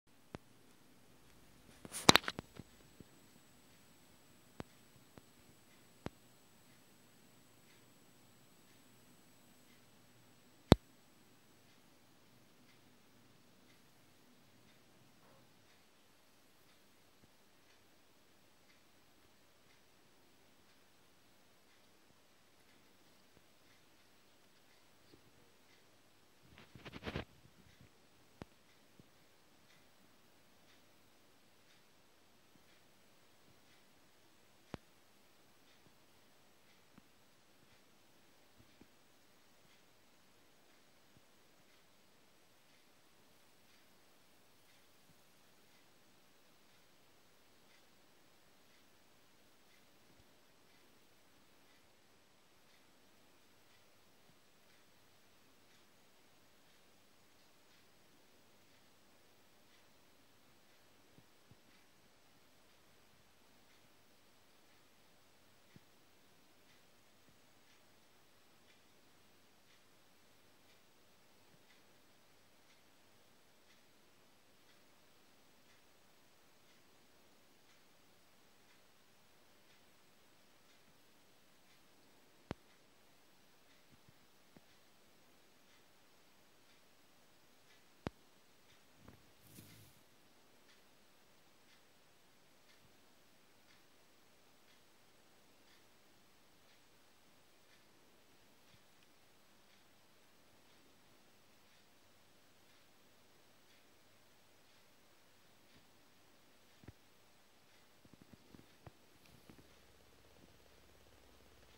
Hobbyking gimbal hand test smooth setting
Good..but roll can shake over 45 degree  forward or backward by the pitch axis.
I used smooth config fileuploaded on HK files tab. Both flashed motor and controller.